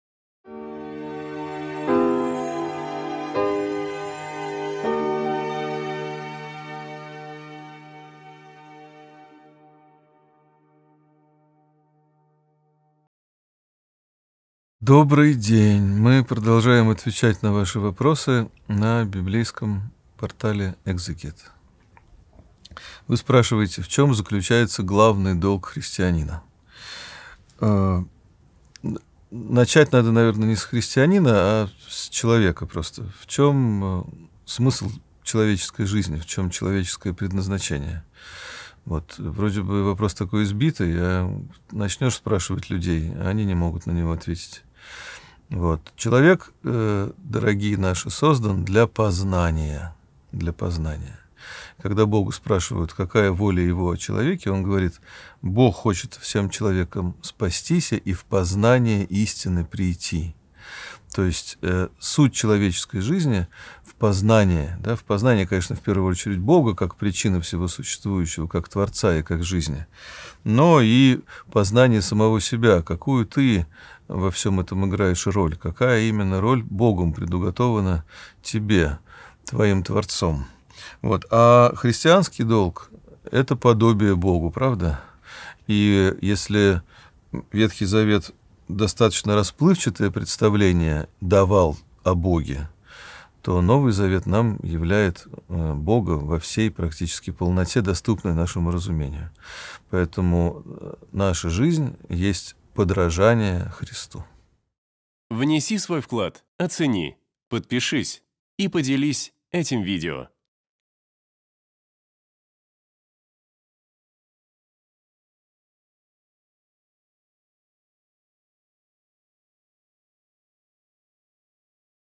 В чём состоит ГЛАВНЫЙ ДОЛГ ХРИСТИАНИНА ? Протоиерей Димитрий Рощин
Команда Экзегет.

В сегодняшнем видео на библейском портале Экзегет со священником Димитрием Рощиным отвечаем на вопрос: "В чем состоит главный долг христианина?"

🔸  Постижение Бога
🔸  Познание самого себя
🔸  Подражание Христу

📜 Читать 1-е Послание к Тимофею главу 2, стих 3 (с толкованиями)